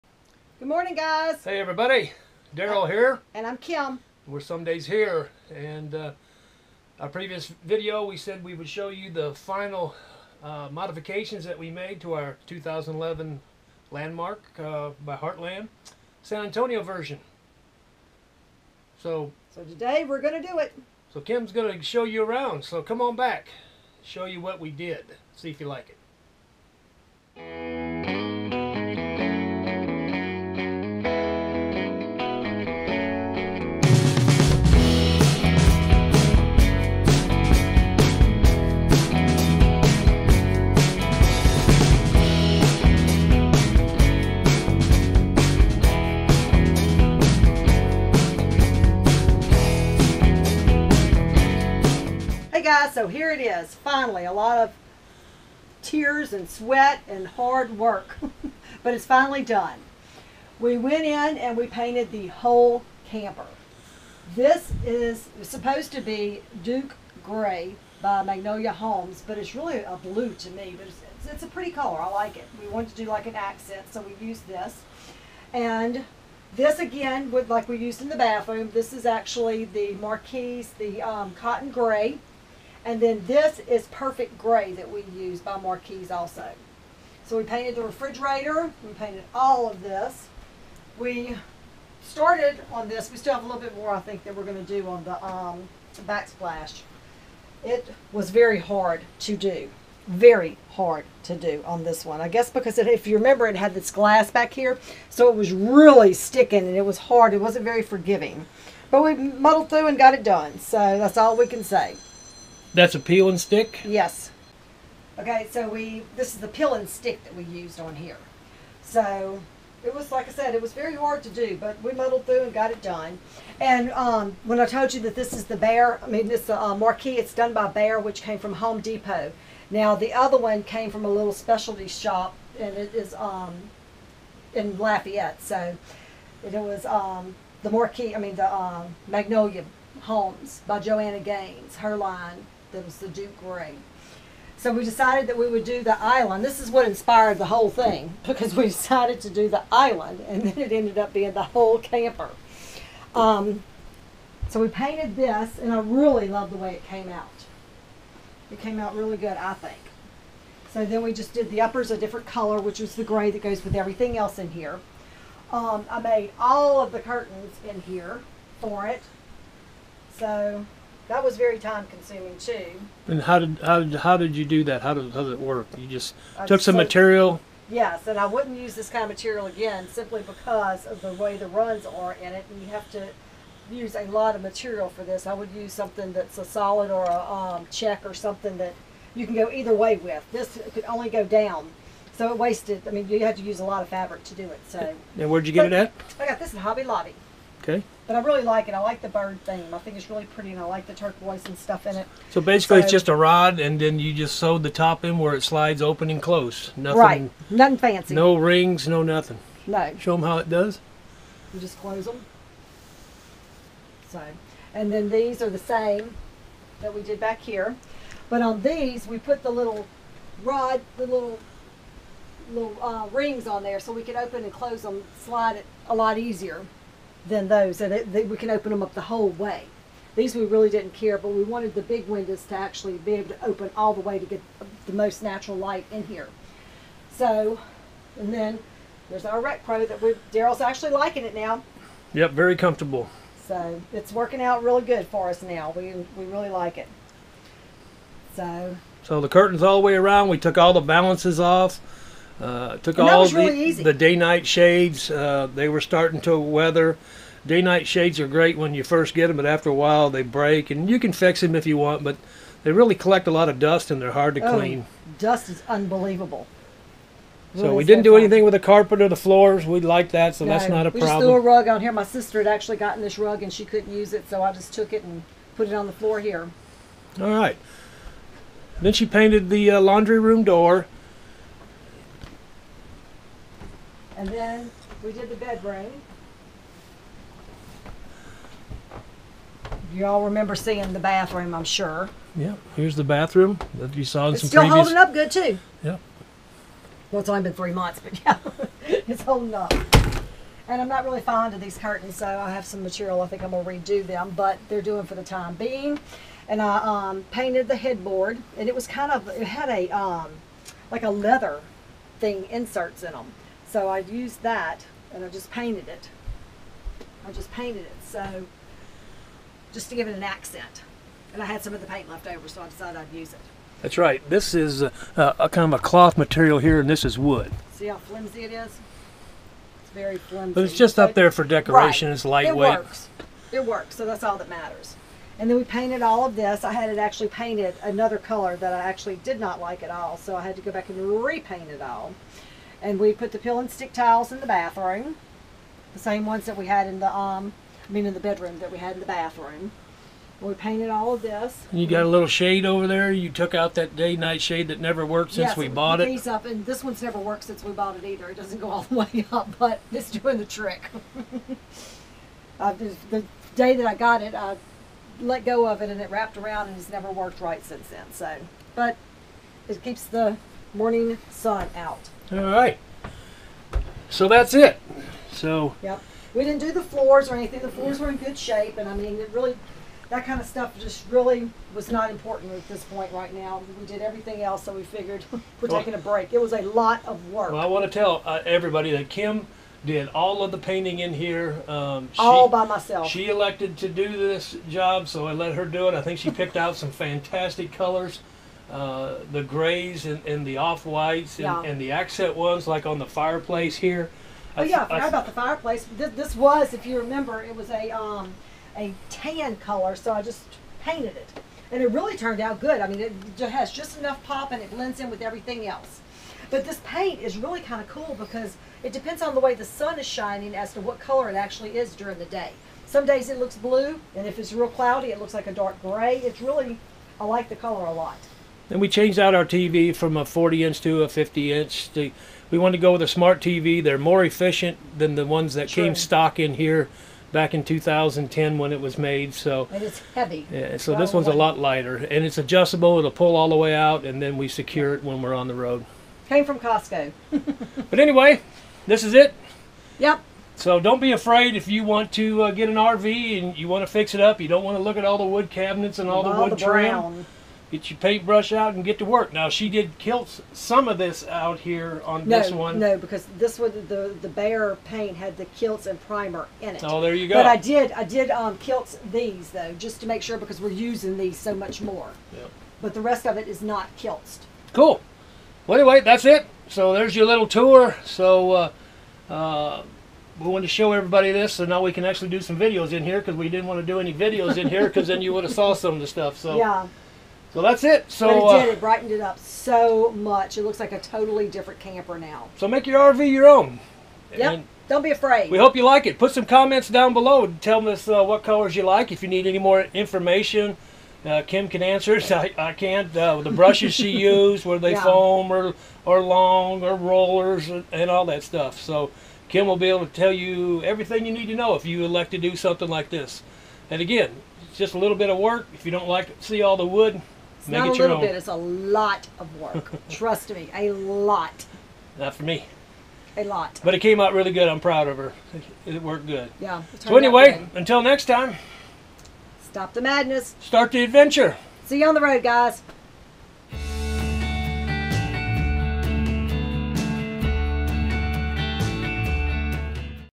RVing Full Time...5th Wheel Remodel Interior
Kim totally went thru the entire interior of this 2011 Heartland Landmark San Antonio version.  She painted almost every wall, made curtains for every window, removed the table and chairs, and installed peel and stick tile to some back/side splashes.